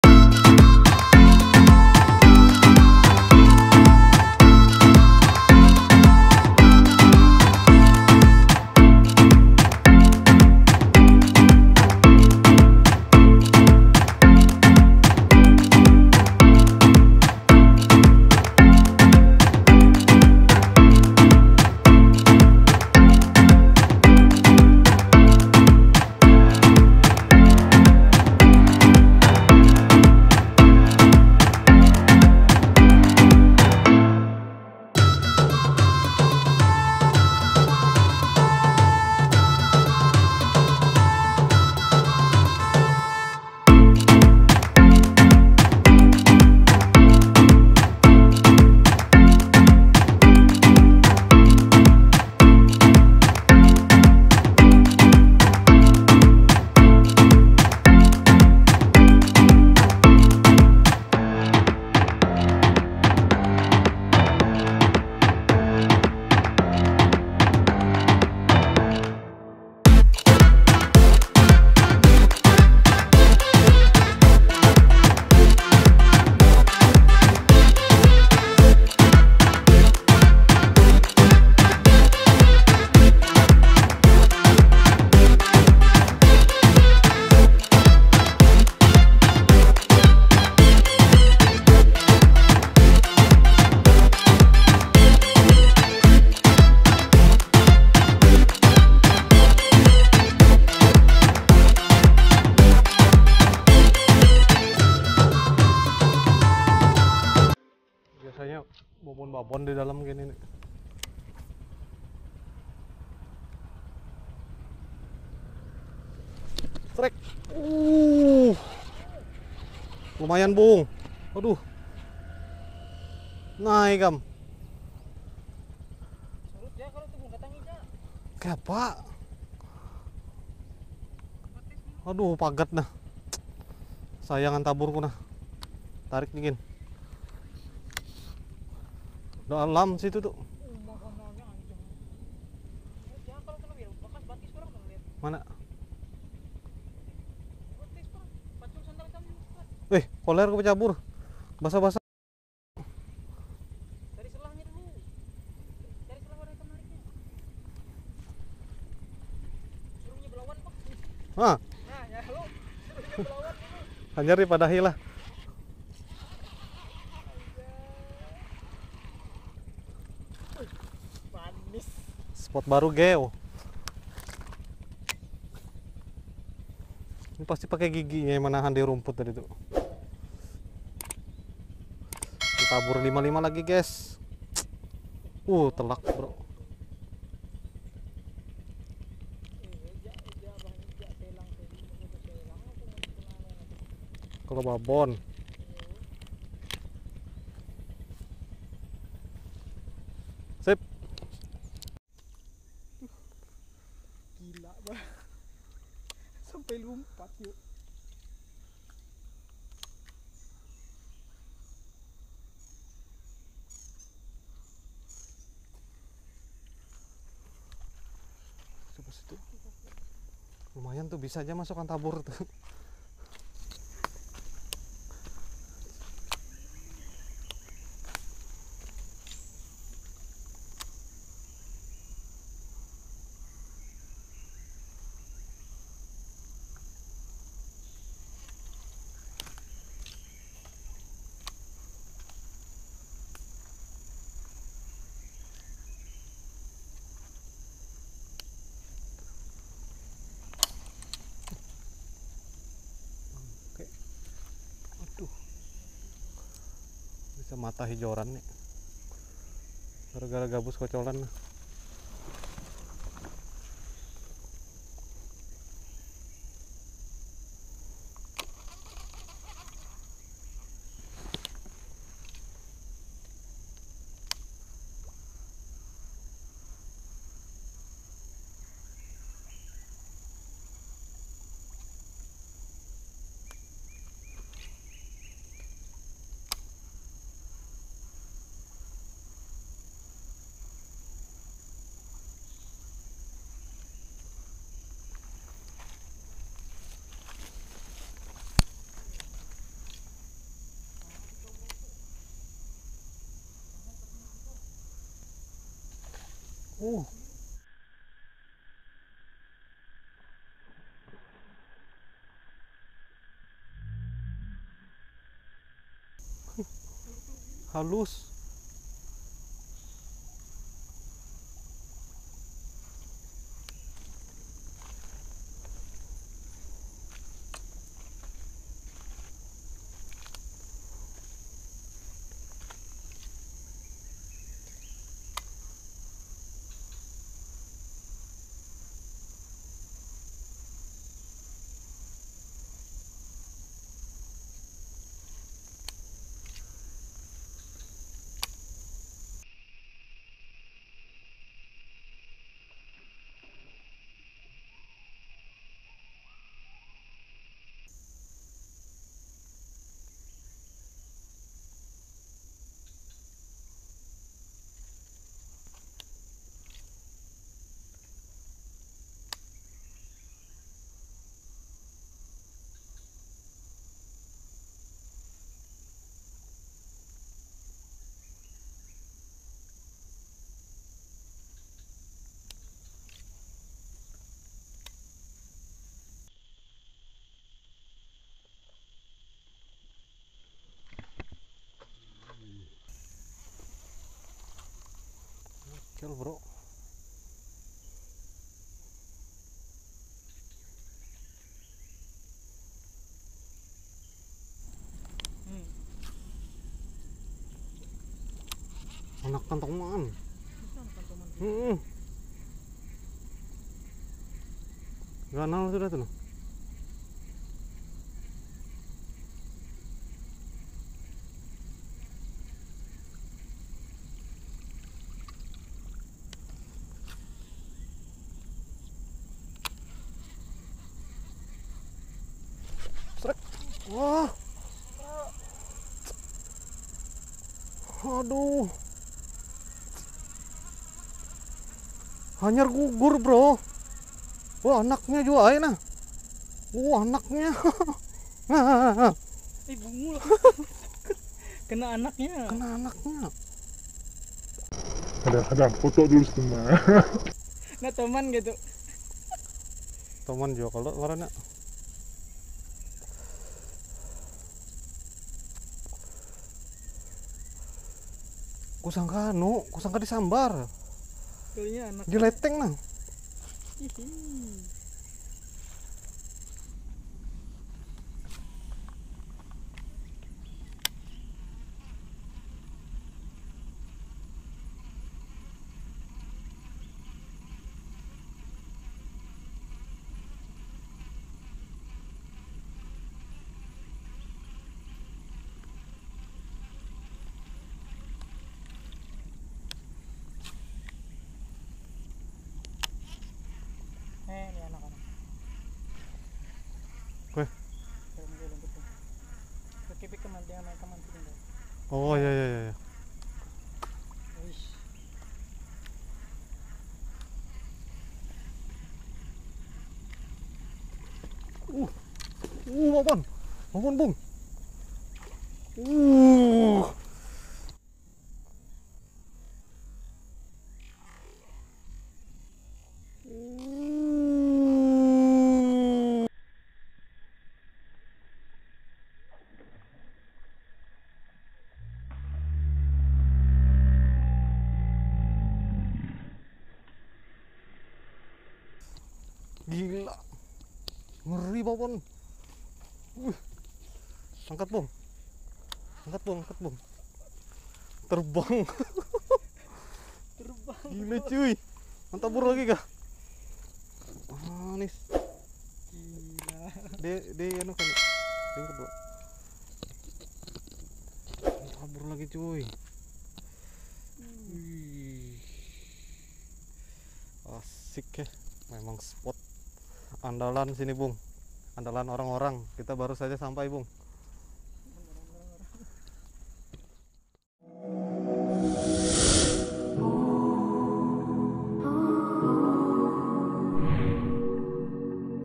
Spot Anti Boncoz ‼️Casting Gabus malah disambar Anakan Toman
Mancing kali ini kita mencoba spot baru ya teman-teman.

Mancing ikan gabus malah disambar anakan ikan toman yang sudah yatim piatu 😂